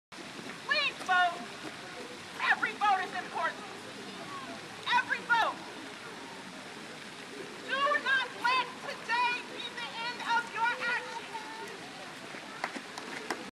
Mary Hill urges Trayvon Martin rally crowd to use your vote.
Please vote says Mary Hill at a rally in solidarity with the family of Trayvon Martin , a florida teen shot and killed while going to the store for candy. Portland rallied March 24 in Peninsual Park.